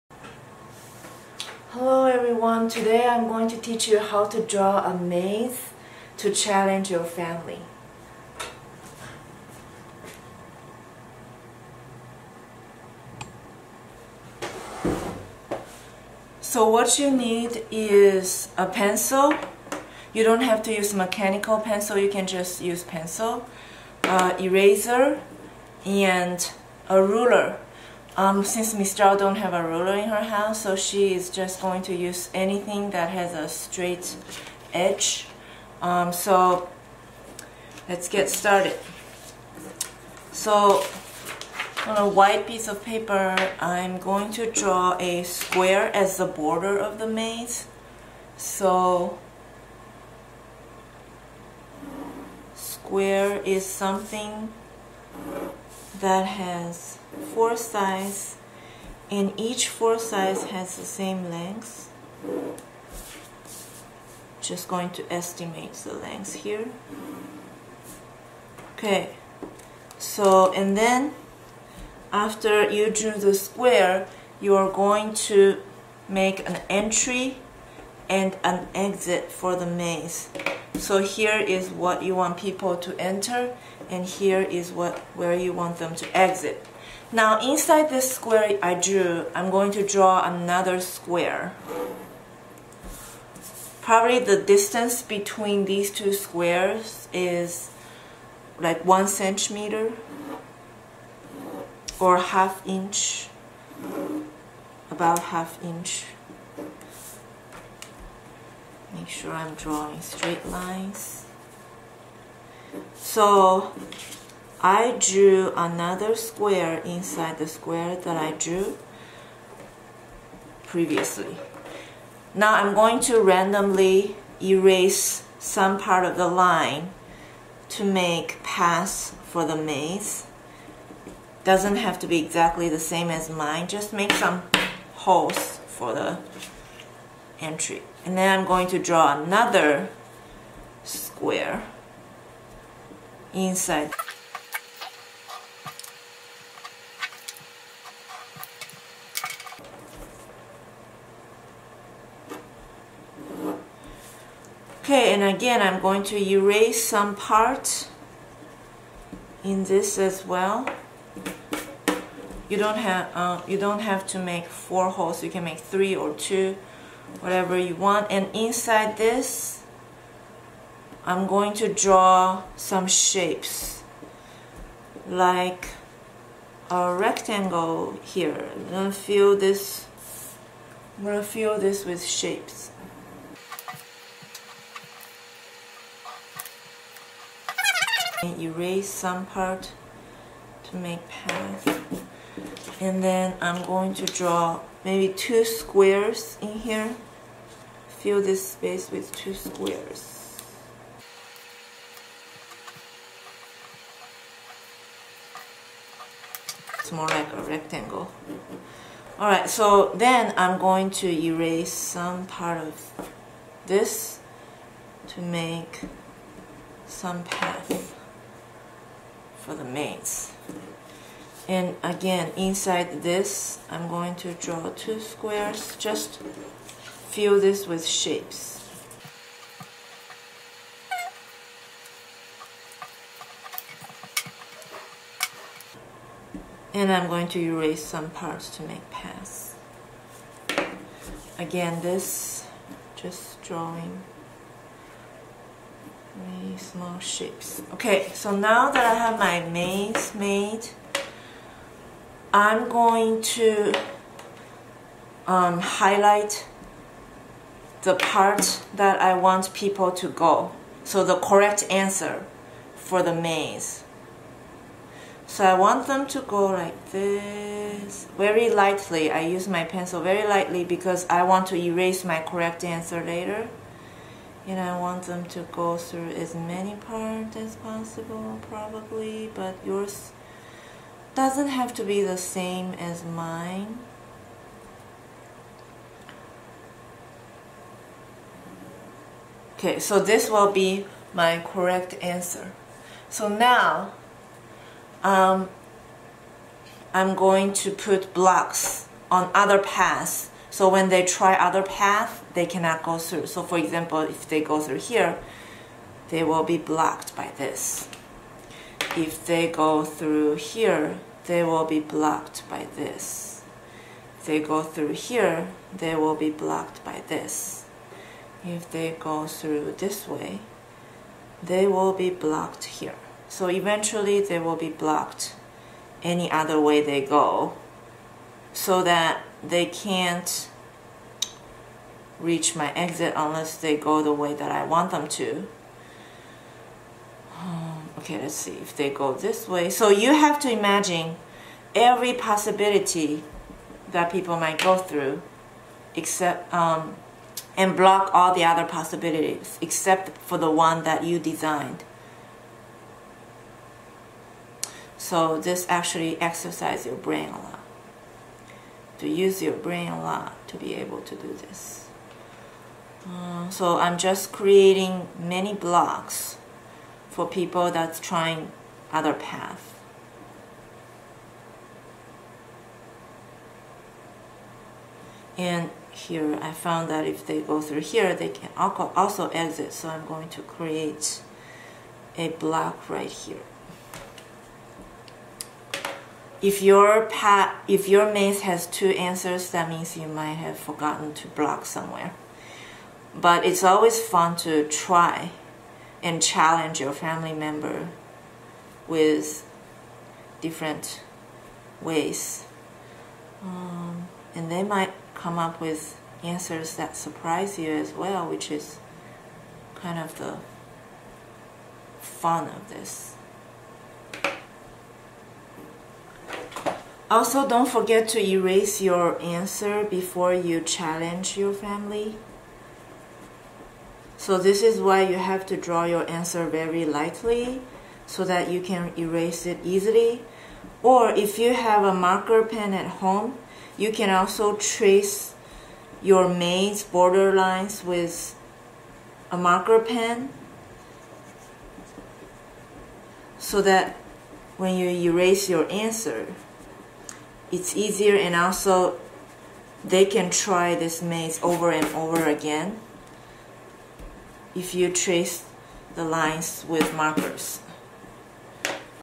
How to Draw a Maze
While home schooling, it could be fun to draw a maze and challenge your family.